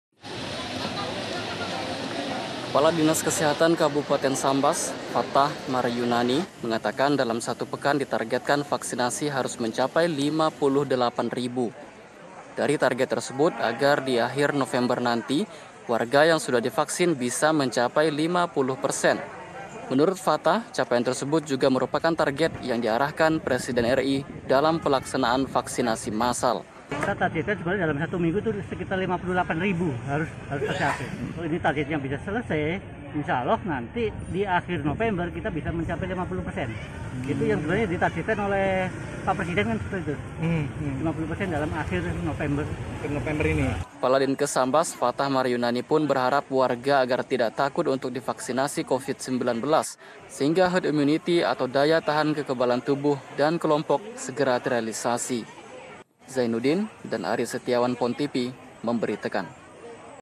VAKSINASI DITARGETKAN 58 RIBU DALAM SEMINGGU
Pemkab Sambas melalui dinas kesehatan menargetkan vaksinasi covid-19 mencapai angka 58.000 setiap pecan. Sehingga dengan target tersebut, pada akhir bulan November nanti, warga yang sudah divaksin bisa mencapai lima puluh persen